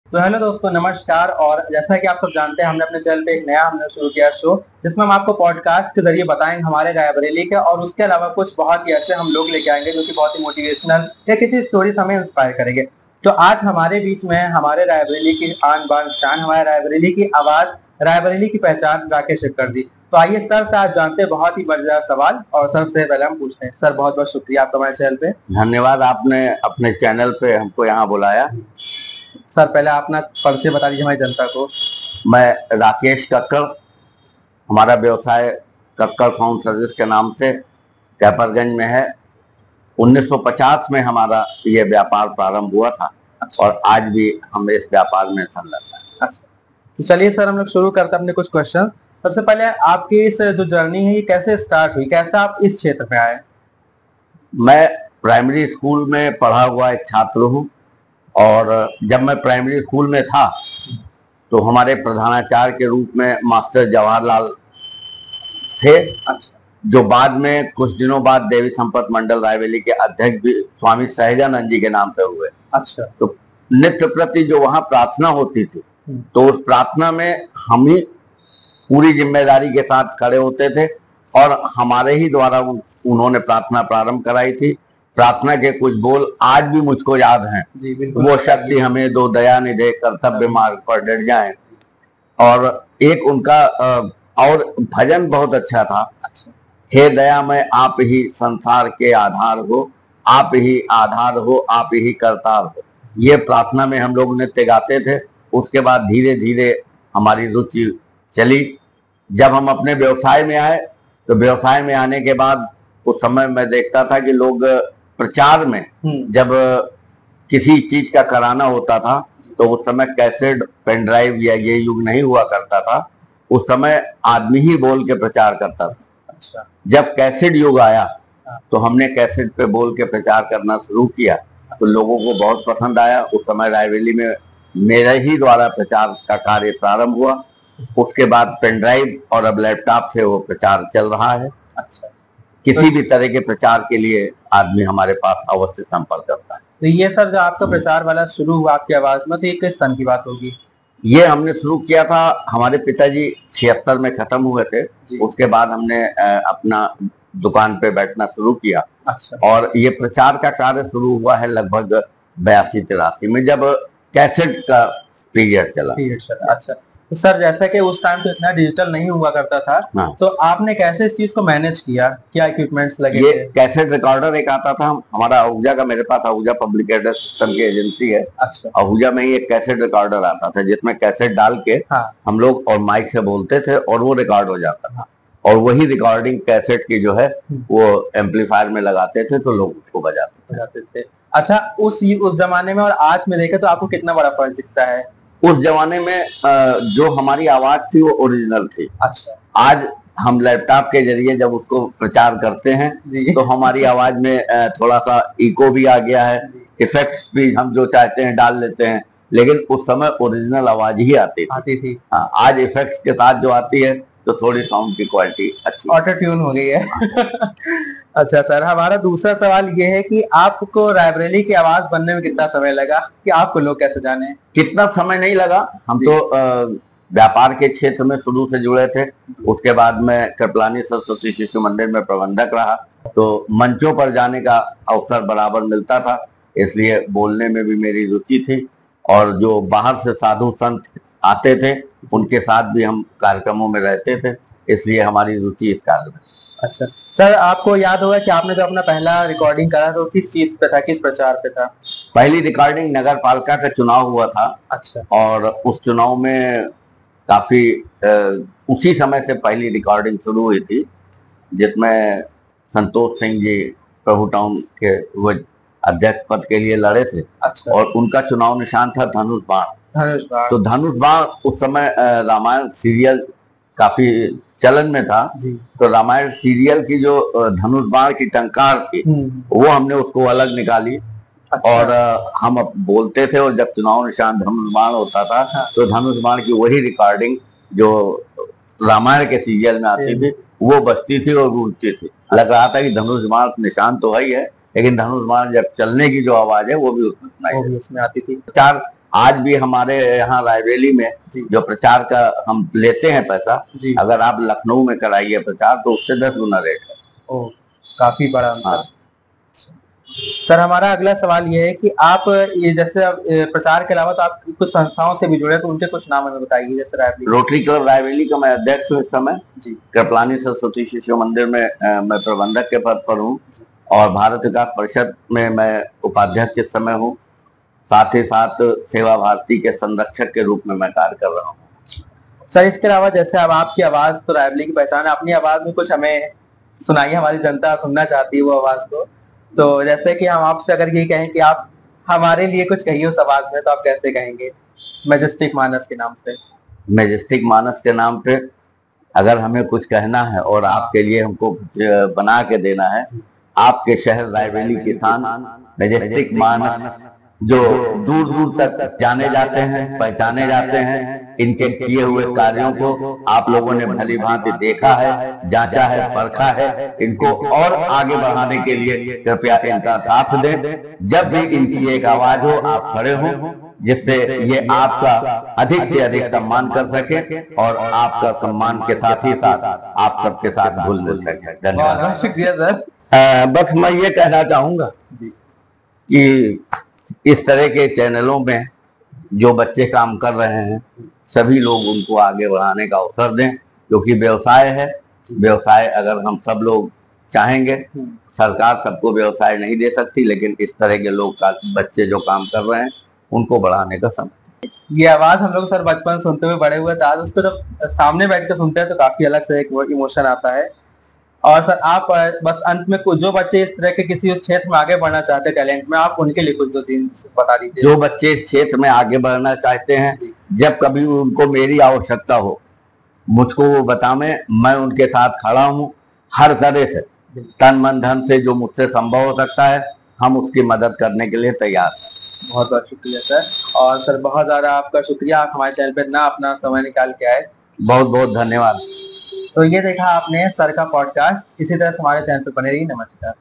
Raebareli की आवाज़ कैसे बने Rakesh Kakkar | राकेश कक्कड़ की जिंदगी बदली उनकी आवाज़ ने | Podcast
Our channel is about VLOGS. We cover lots of cool stuff